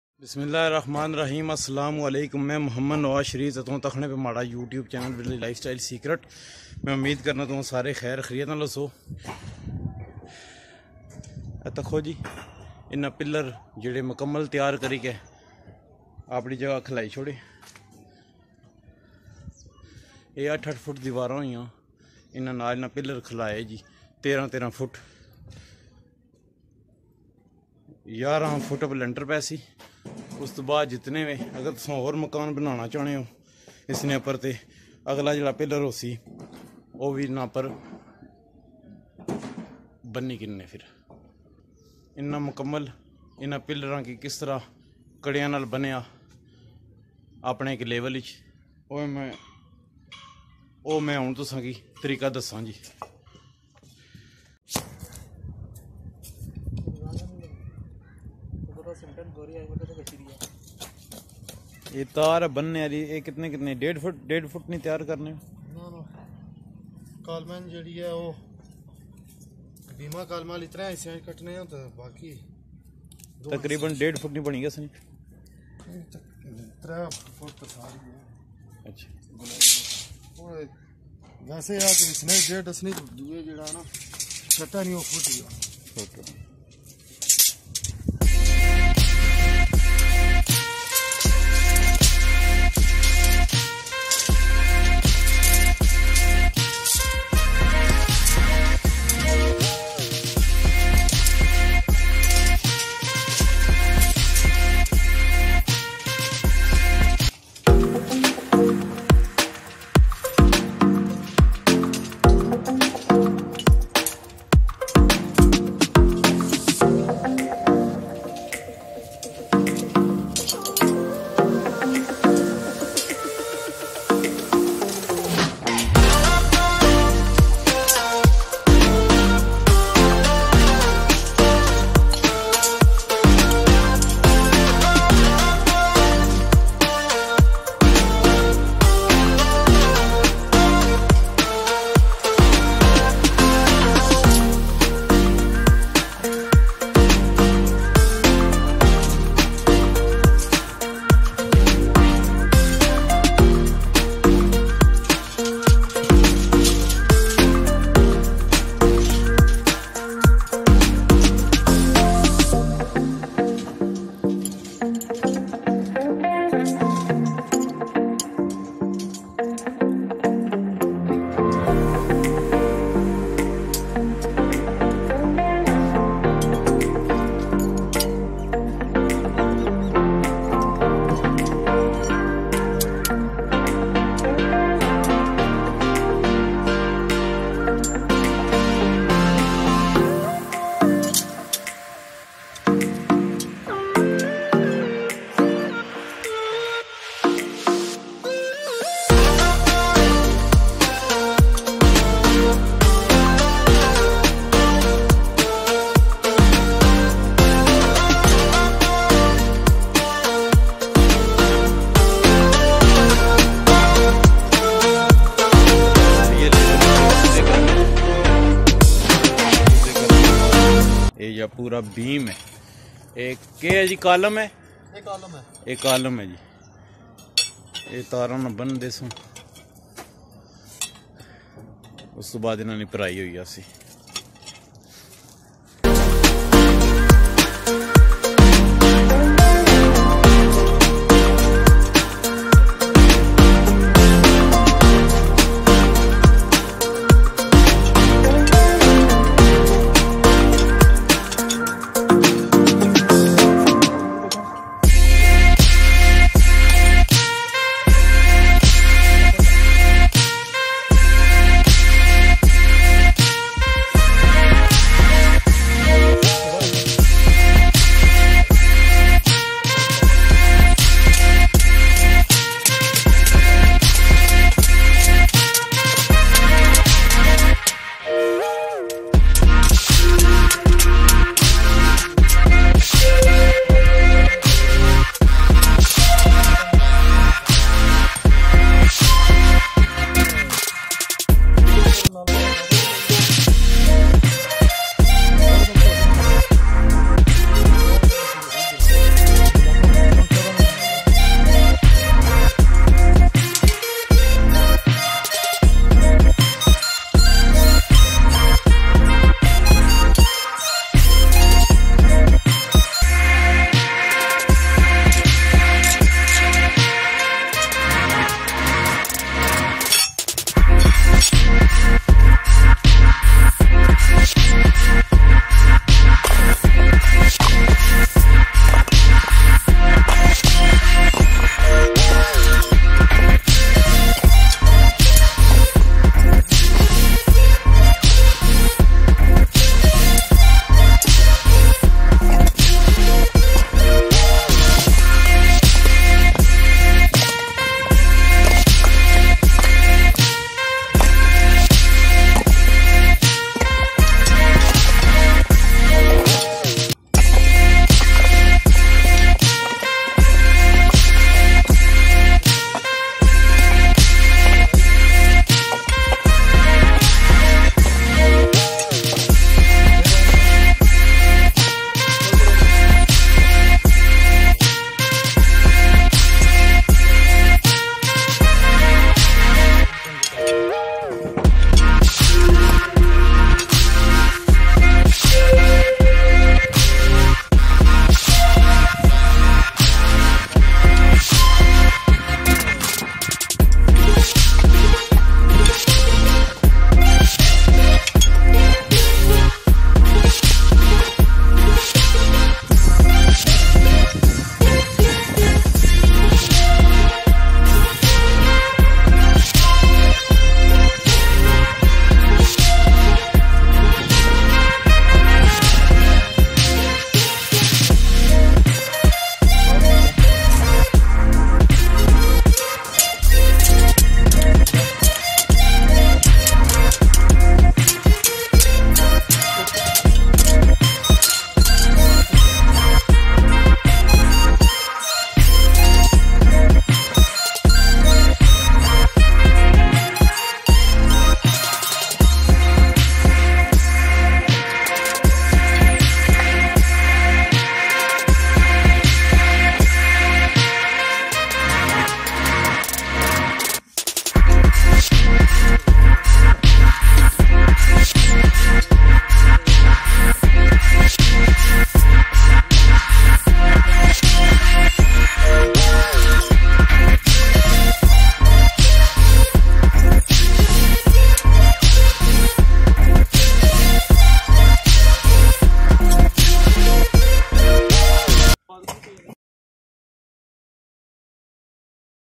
How to make RCC column for House./ Village lifestyle secrets / لوے کے کالم بنانے کا طریقہ /
لوے کے کالم بنانے کا آسان طریقہ نواز شریف کے ساتھ؛ 5مرلہ گھر کے 12 کالم